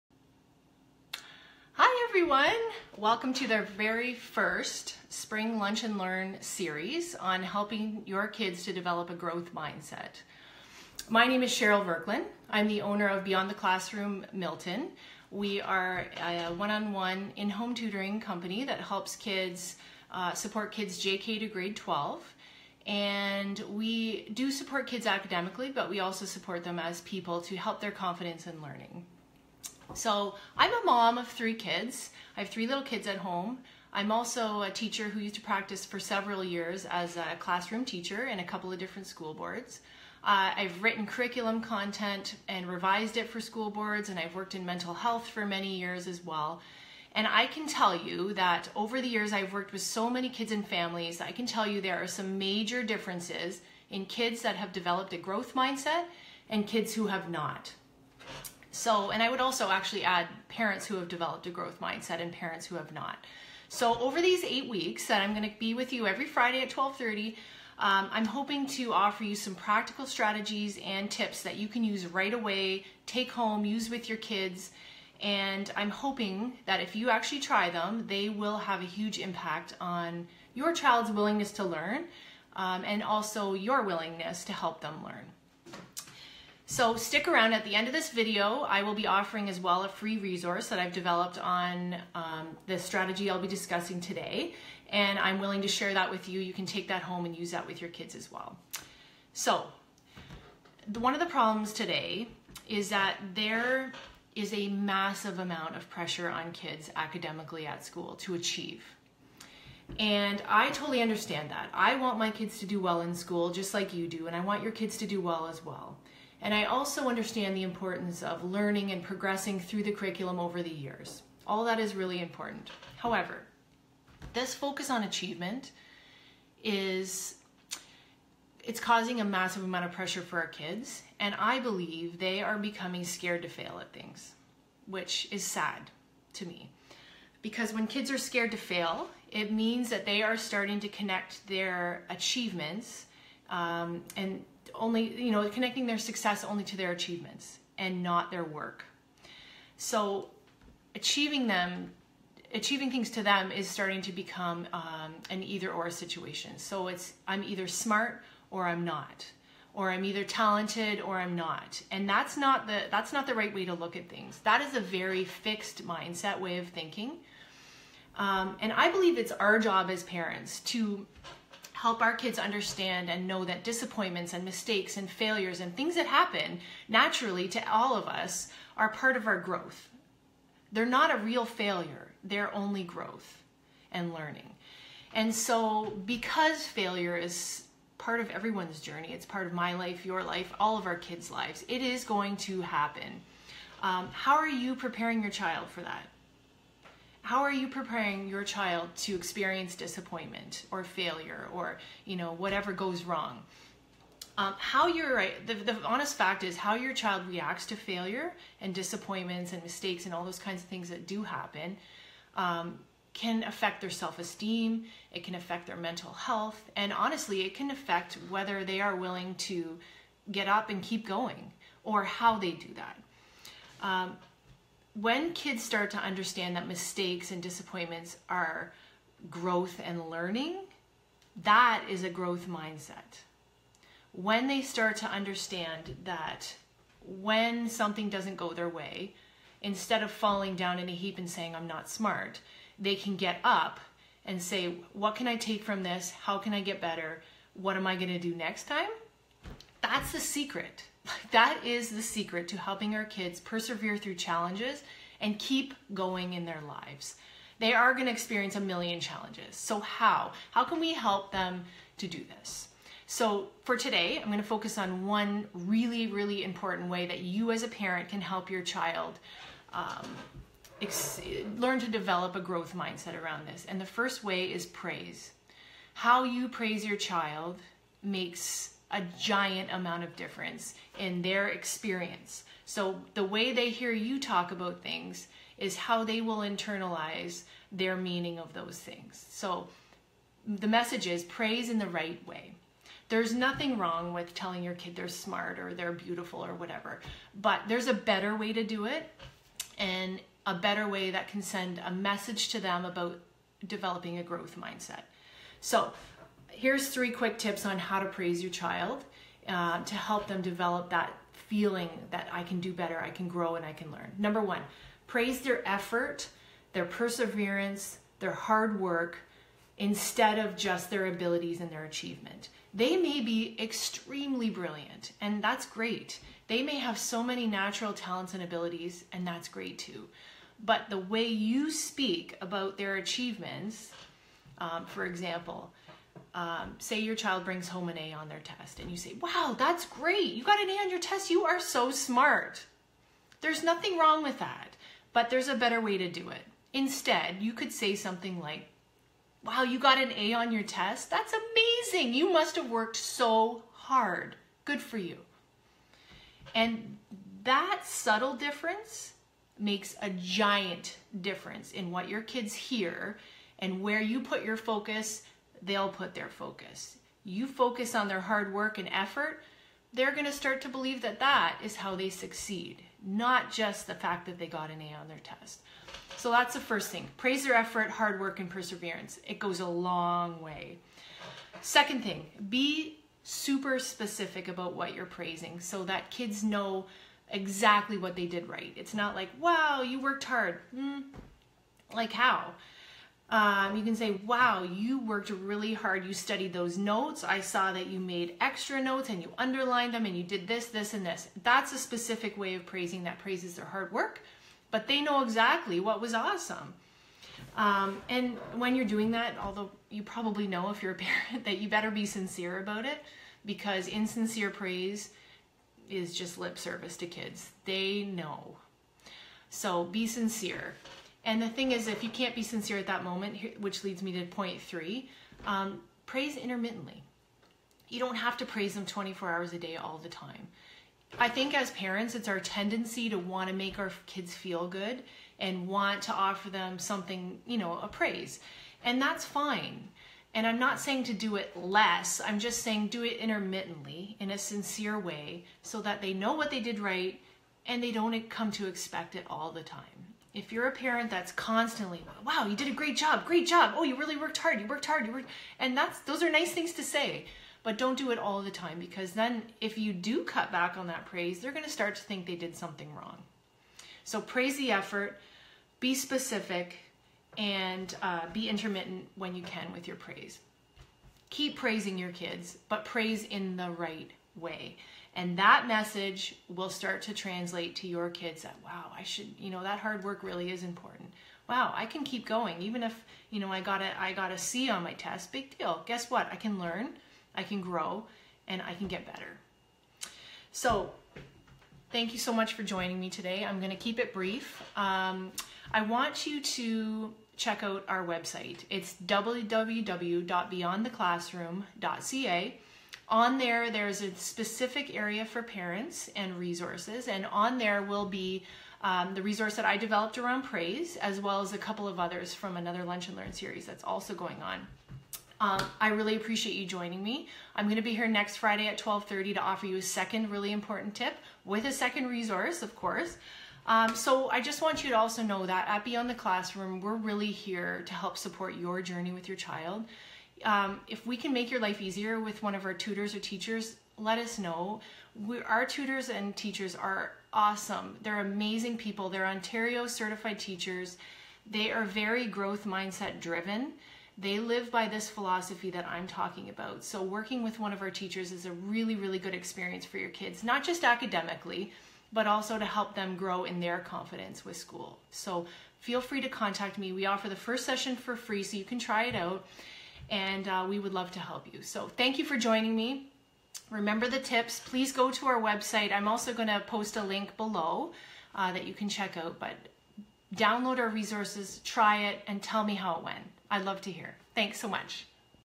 Raising Kids with a Growth Mindset: Praise in all the Right Ways!
Cheryl Vrkljan, owner of Beyond the Classroom Milton talks about cultivating a growth mindset in our kids by the use of praise.